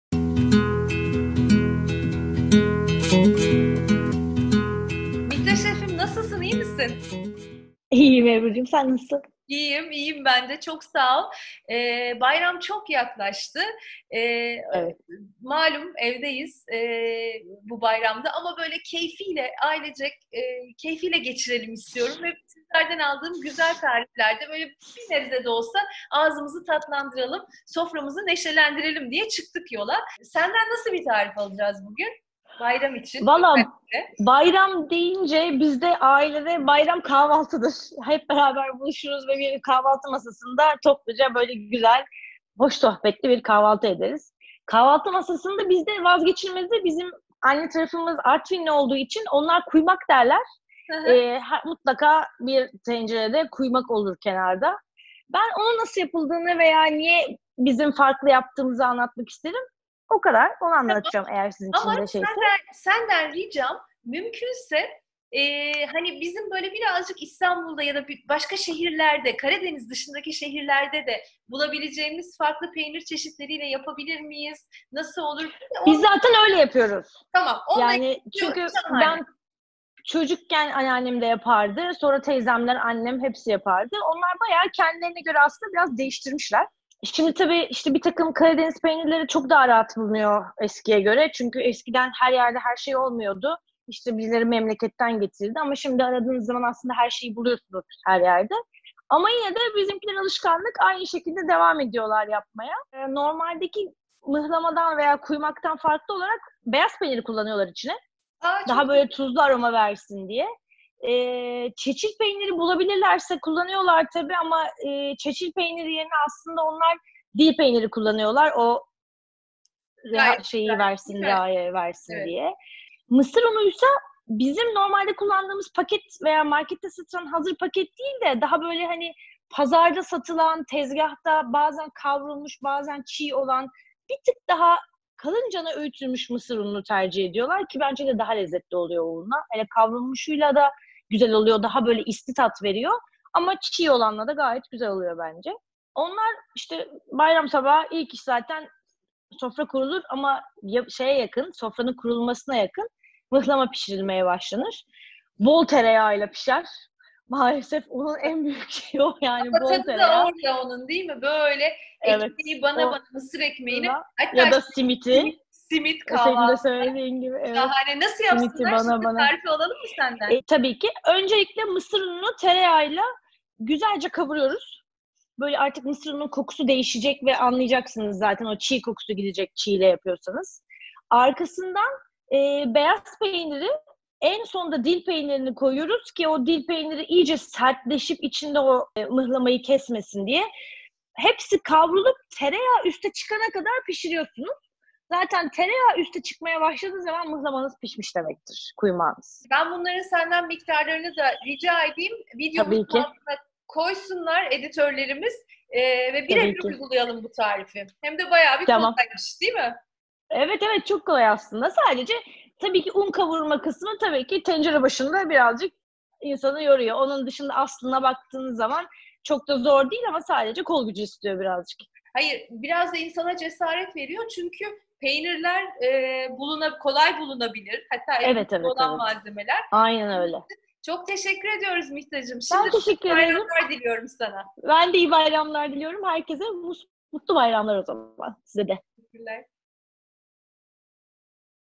Şef Mihta Yıldırımtaş Bayram Sofrasına Özel Kuymak Tarifi Veriyor
Hürriyet Pazar gastronomi yazarı Ebru Erke'nin 'bayram sofrası' kuruluyor. İlk öğün olan kahvaltı için tarif Mihta Yıldırımtaş'tan: Kuymak! Üstelik her yerde bulunabilecek malzemelerle... Mihta Şef, “Bizde bayram sofrası demek uzun kahvaltı sofraları demektir” diyerek Artvinli anneannesinin tarifini paylaştı: 100 gram mısır ununu (varsa kalın çekilmiş ve kavruk olanından kullanın) 150 gram tereyağında kokusu hafif değişene kadar kavurun. 250 mililitre süt ekleyip karıştırın ve hemen ardından önce 150 gram beyaz peynir, o eridikten sonra da varsa çeçil yoksa 100 gram dil peyniri ekleyin. Akışkan bir kıvam alana kadar karıştırmaya devam edin. Tereyağı üste çıkınca kuymağınız pişmiş demektir.